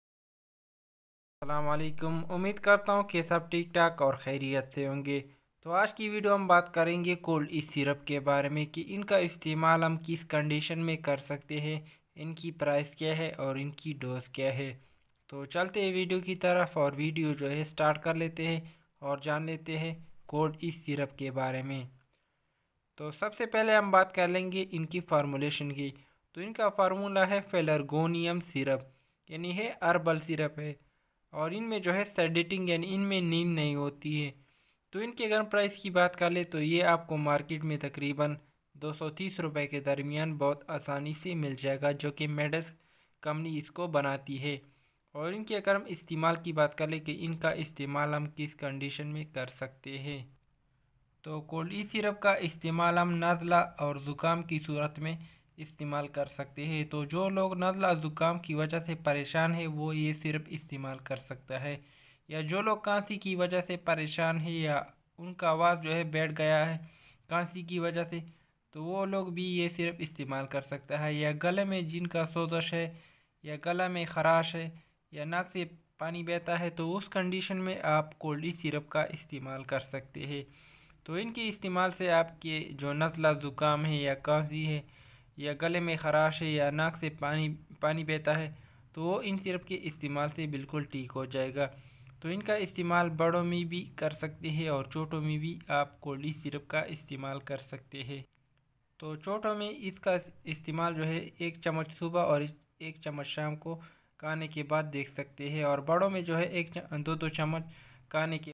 Natural Remedy For Cold & Flu !! How To Use Coldeez Syrup ( Pelagronium ) Urdu Flu Cold children
Natural Remedy For Cold & Flu !! How To Use Coldeez Syrup ( Pelagronium ) Urdu